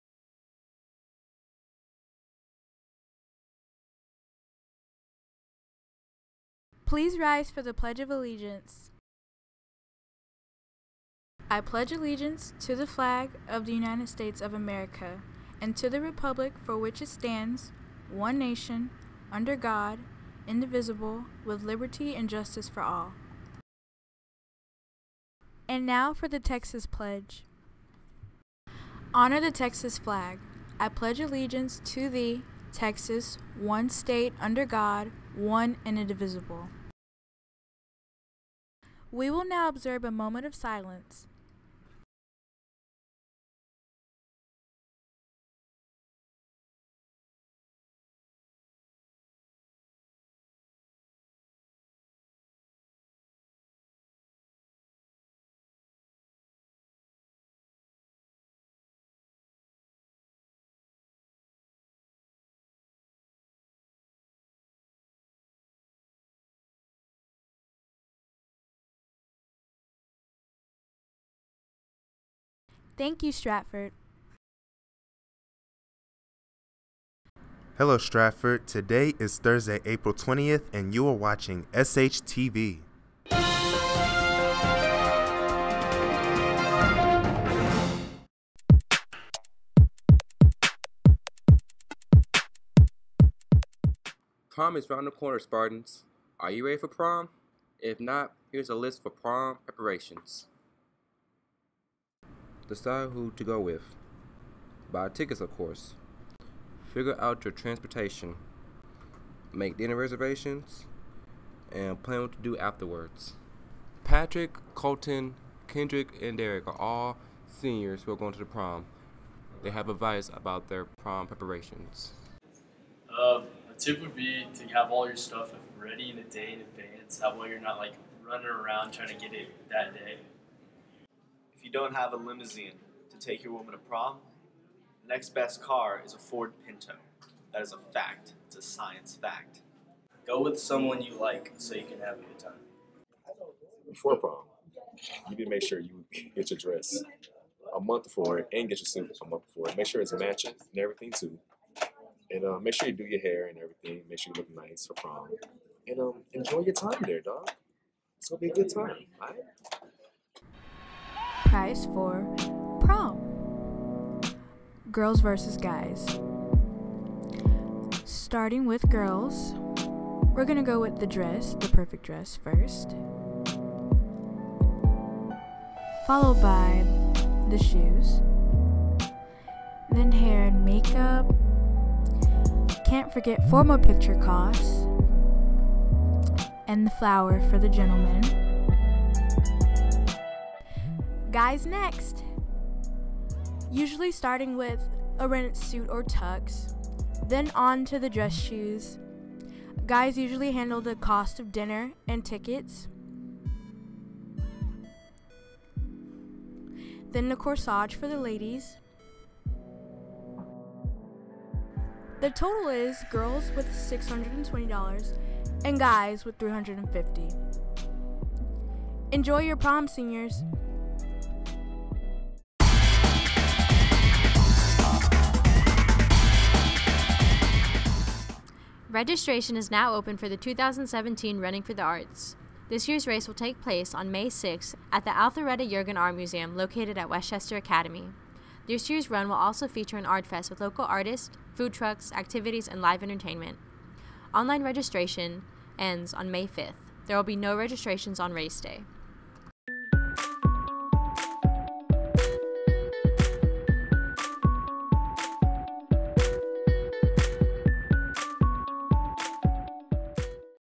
SHTV Thursday Show: April 20, 2017
In this episode, we'll be covering prom preparations and the costs of it for boys and girls.